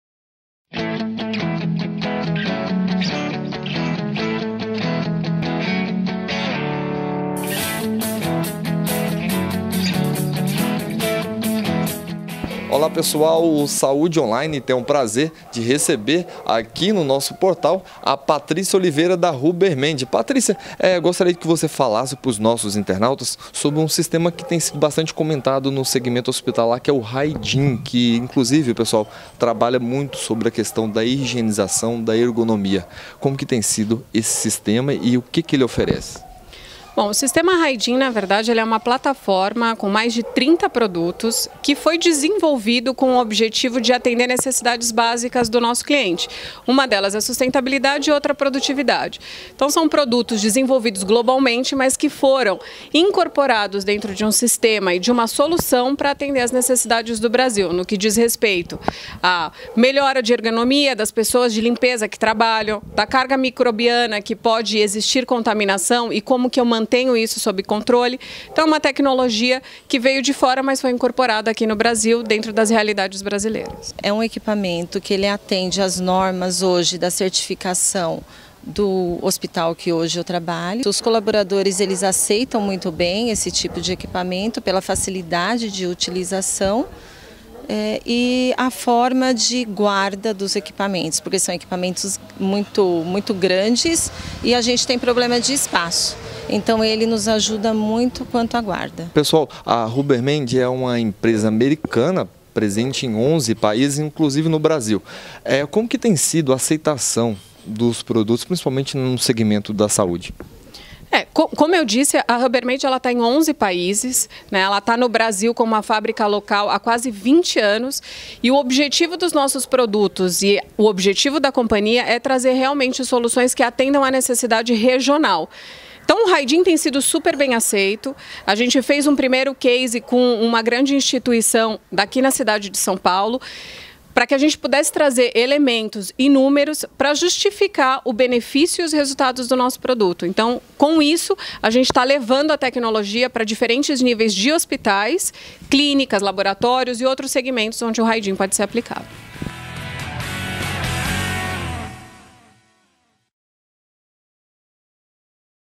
Sistema Hygen da Rubbermaid melhora a produtividade de colaboradores
Equipamento pioneiro e tecnológico de higiene profissional é composta por mais de 30 produtos e tem como principal diferencial a performance de limpeza comprovada.